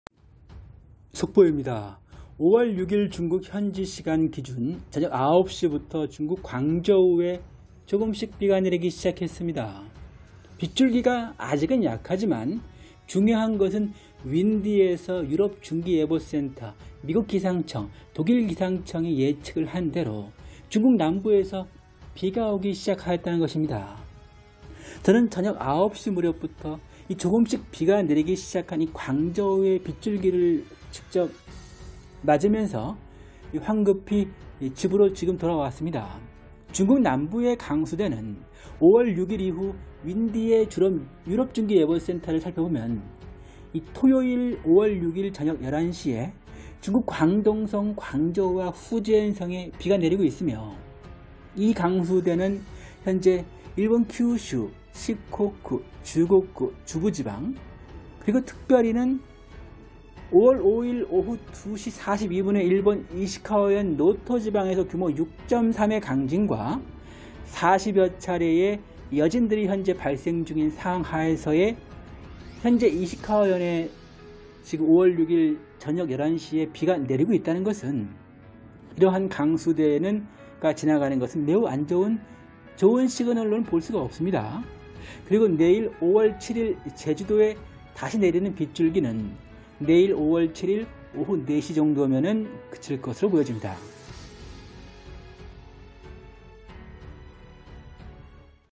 5월6일 저녁 9시 중국 남부 폭우 일본 이시가와현 폭우 40여차례 지진 5월7일 제주도 비소식
5월6일 저녁 9시 중국 남부  폭우 일본 이시가와현 폭우 40여차례 지진 5월7일 제주도 비소식